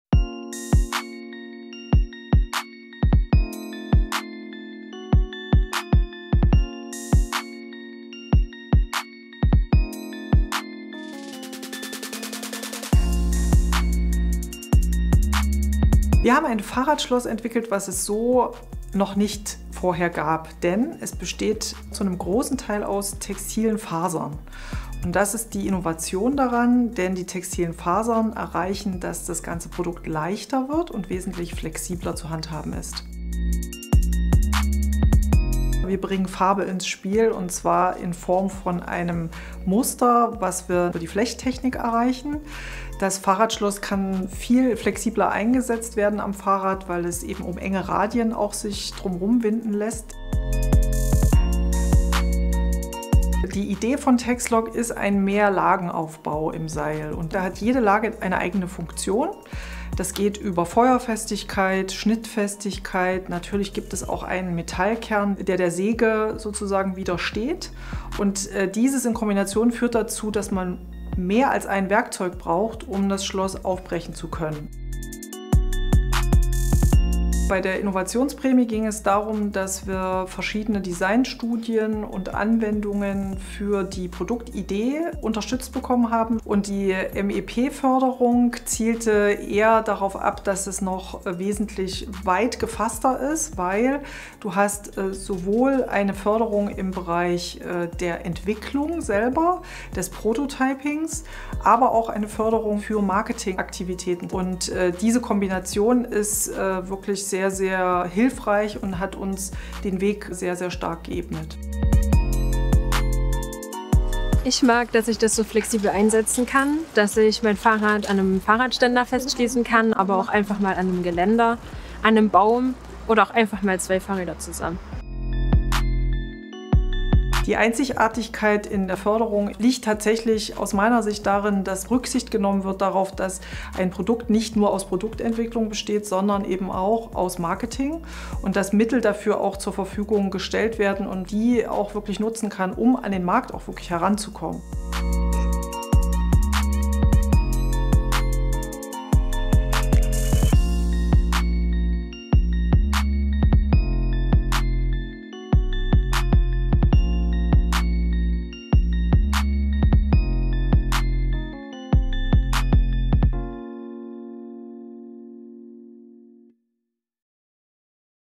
Markteinführung innovativer Produkte: Texlock GmbH, Leipzig
Tex-Lock stellt flexible und sichere Fahrradschlösser her, die aus verschiedenen Lagen von Textilfasern zusammengesetzt sind. Das Schloss ist in dieser Art einzigartig und ermöglicht es das Rad je nach Bedarf an einem Fahrradständer, Geländer oder Laternenpfahl anzuschließen.
Das EFRE-Förderprogramm unterstützt Unternehmen ihre innovativen Ideen zur Marktreife zu bringen bzw. Prototypen marktfähig zu machen und auf dem Markt zu etablieren.